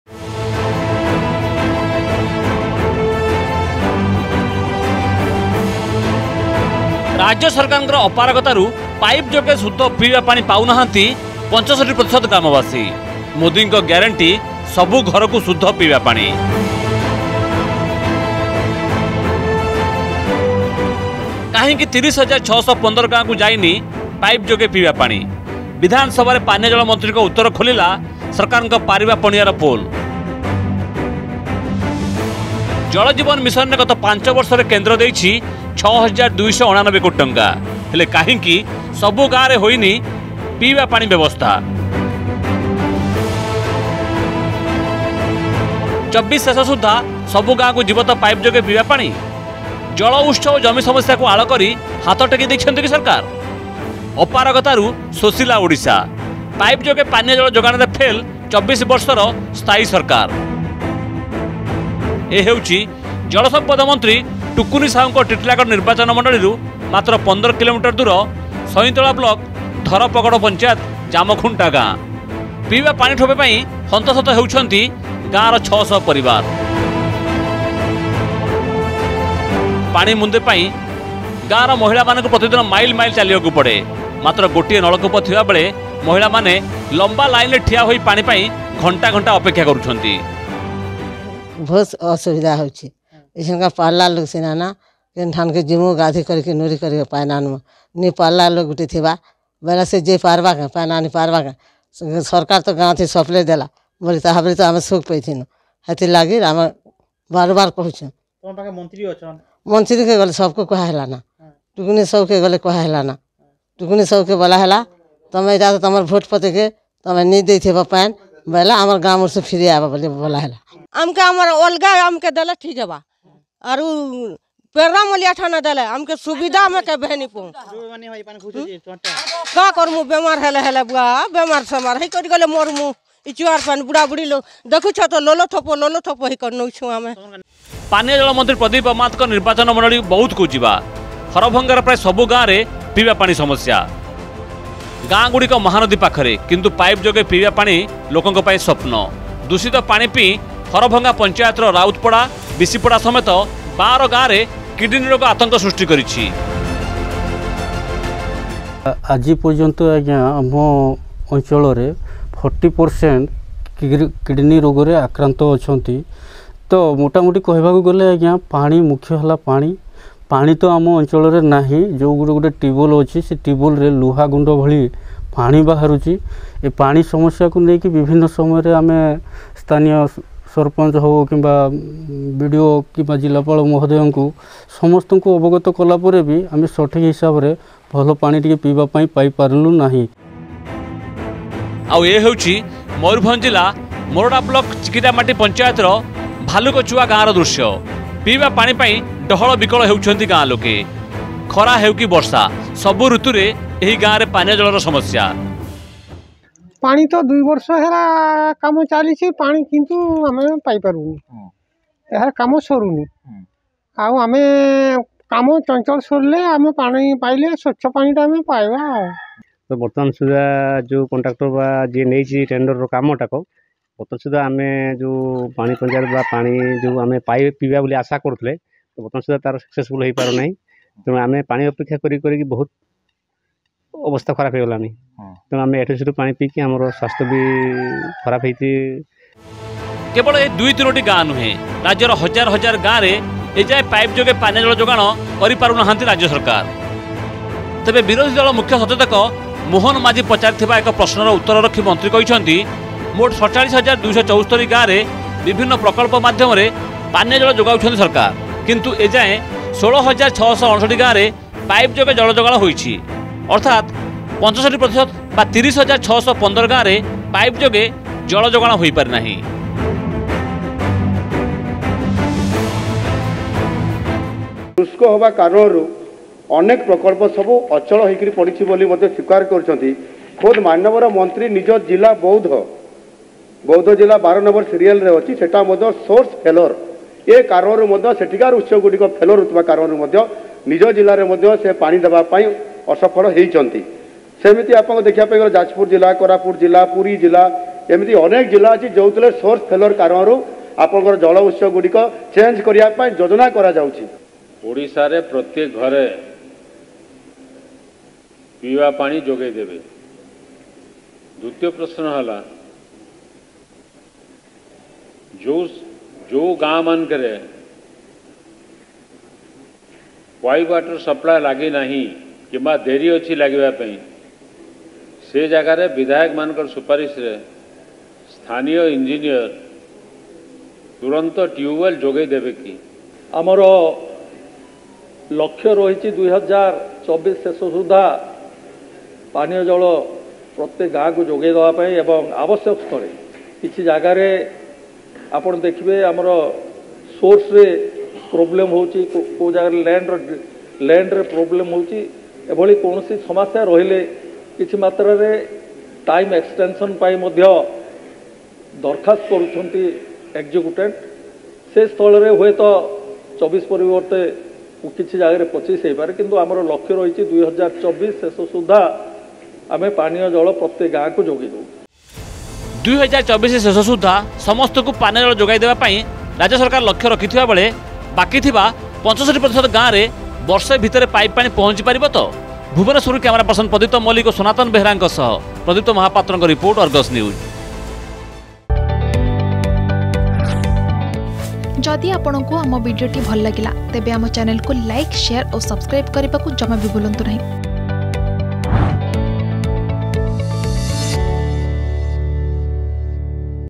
ପାନୀୟ ଜଳ ମନ୍ତ୍ରୀଙ୍କ ଉତ୍ତର ଖୋଲିଲା ବିଜେଡି ସରକାରଙ୍କ ପାରିବା ପଣିଆର ପୋଲ୍ || Pure Drinking Water Issue
ବିଧାନସଭାରେ ପାନୀୟ ଜଳ ମନ୍ତ୍ରୀଙ୍କ ଉତ୍ତର ଖୋଲିଲା ବିଜେଡି ସରକାରଙ୍କ ପାରିବା ପଣିଆର ପୋଲ୍। ଏବେବି ପାଇପ ଯୋଗେ ପିଇବା ପାଣି ପାଉନାହାଁନ୍ତି 65 ପ୍ରତିଶତ ଗ୍ରାମବାସୀ। ଦେଖନ୍ତୁ ଏହି ରିପୋର୍ଟ ।